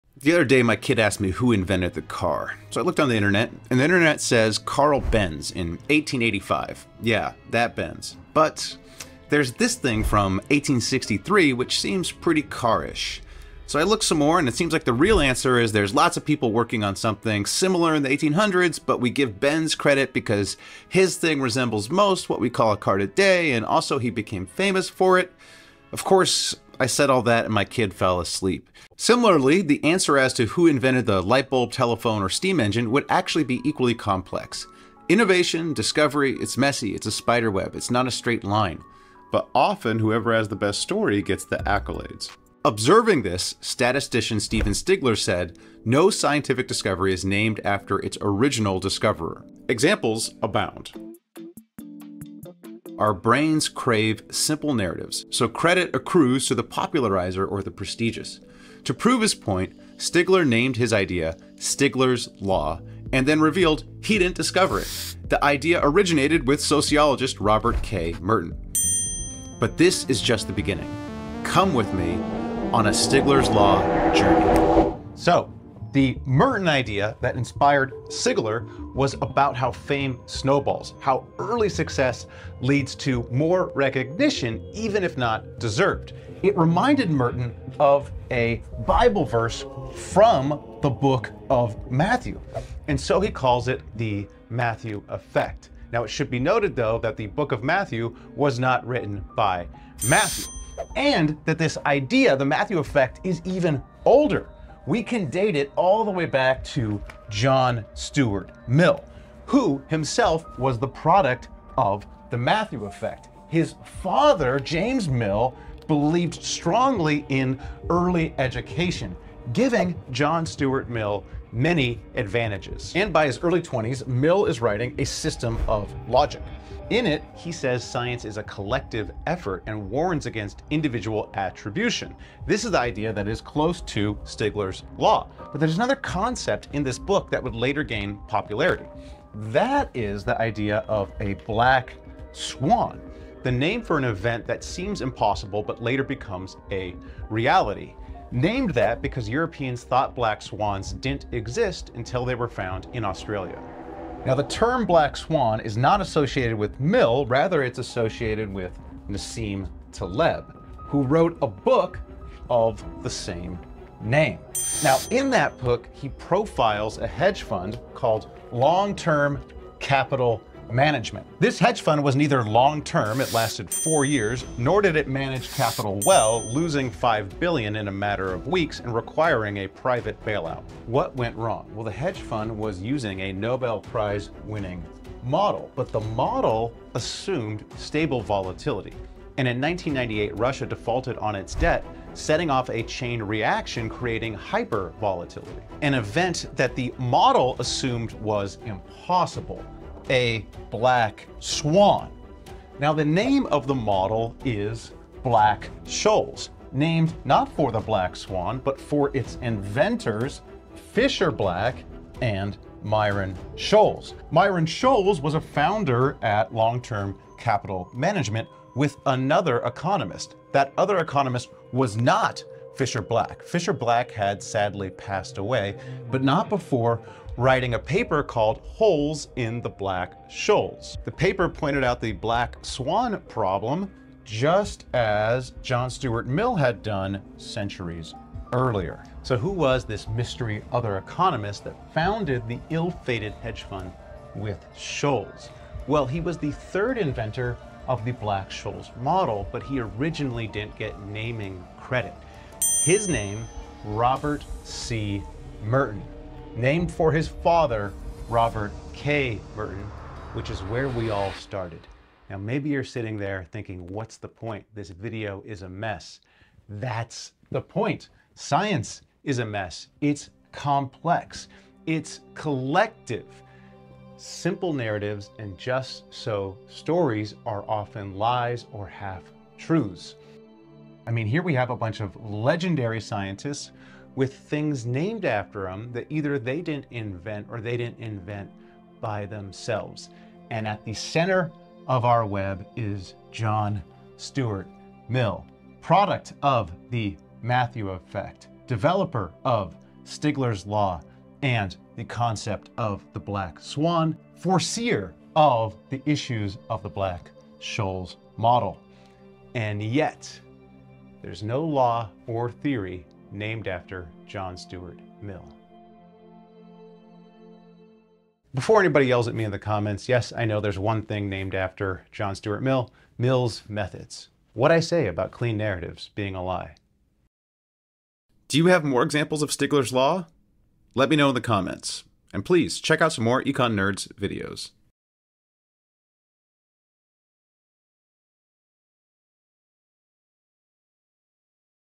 THIS THEORY EXPLAINS THEORIES: Stigler’s Law
Statistician Stephen Stigler argued that “No scientific discovery is named after its original discoverer.” He liked the idea so much that he called it Stigler’s Law (with tongue firmly in cheek). Stigler’s Law is the rare claim that proves itself, because it had previously been made years earlier by another scientist...but hadn't gotten much attention at the time.

In this very messy episode of Econ Nerds, we follow a tangled web of misplaced credit, social prestige, and historical irony, showing how the human preference for simple narratives distorts how we remember innovation.

Bayes’ Theorem wasn’t just Bayes, so update your priors.
Halley’s Comet? Edmond’s pal Isaac (Newton) supplied the math.
The Gaussian distribution? Compared to Gauss, de Moivre was ahead of the curve.

And it only gets messier.

You’ll meet:

Robert K. Merton, the sociologist behind the Matthew Effect (named after the Gospel that probably wasn't written by the Apostle), who missed out on naming credit for Stigler’s Law.

Myron Scholes, the economist whose semieponymous Black-Scholes model powered the world’s most misnamed hedge fund, which was brought down by a black swan event (no relation to the name of the model).

And John Stuart Mill, who wrote about both black swans and the perils of oversimplified scientific credit, but somehow has no “Mill’s Law” to show for it.